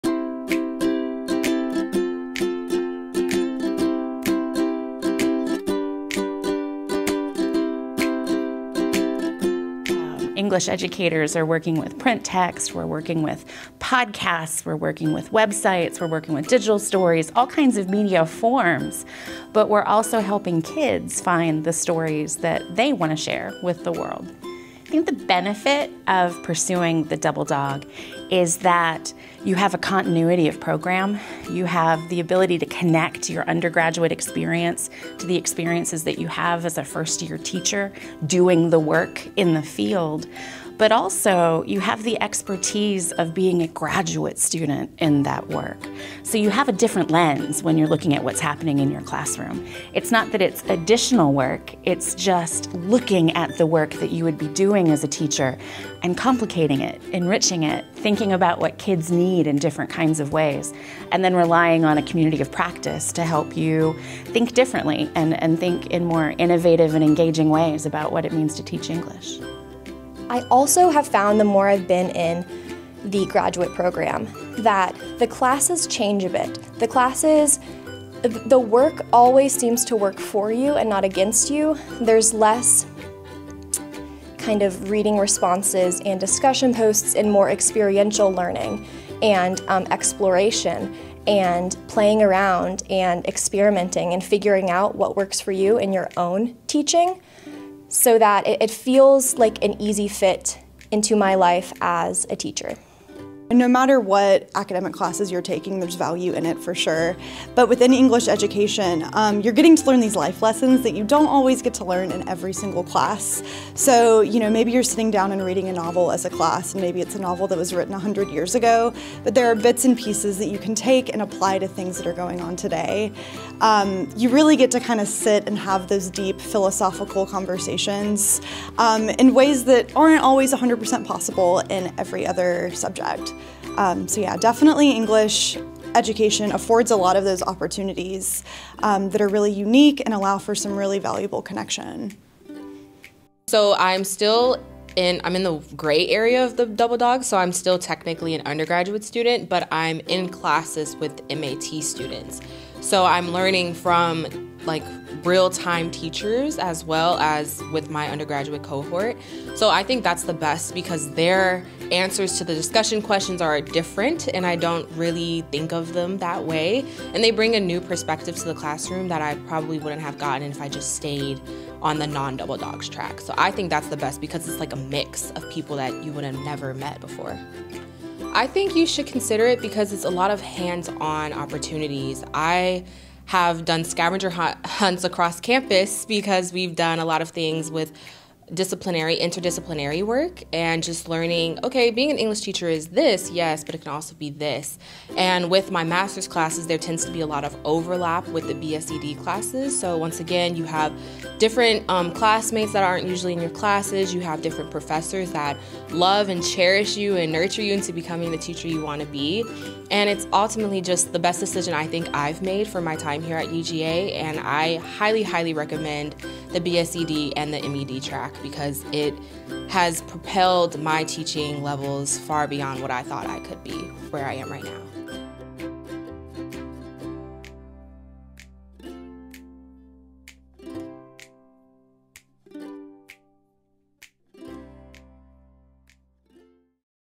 English Education Double Dawgs Program at the Mary Frances Early College of Education at UGA
Prepare for a career teaching English in a Double Dawgs program at the Mary Frances Early College of Education at UGA.  Earn two degrees in five years.